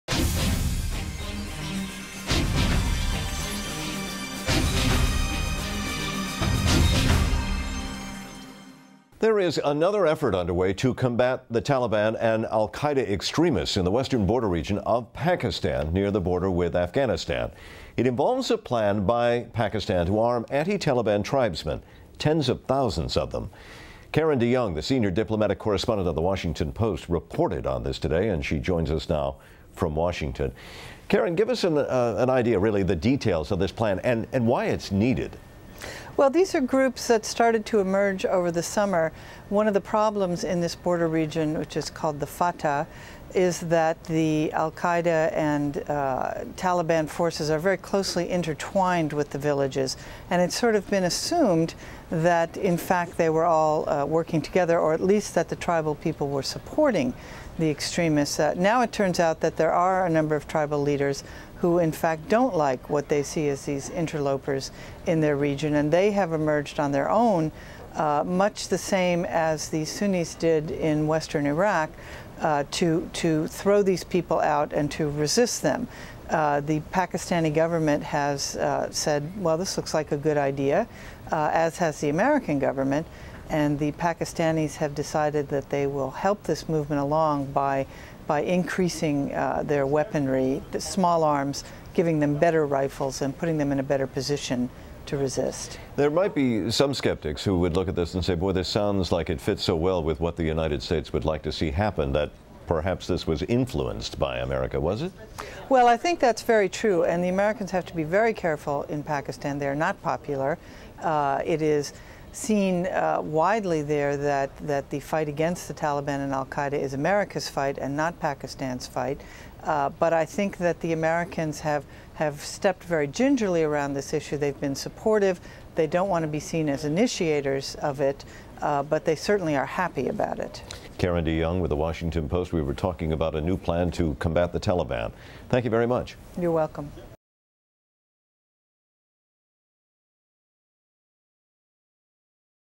Pakistan arms tribesmen to fight Taliban
Karen DeYoung of The Washington Post discusses Pakistan's new plan to fight extremists.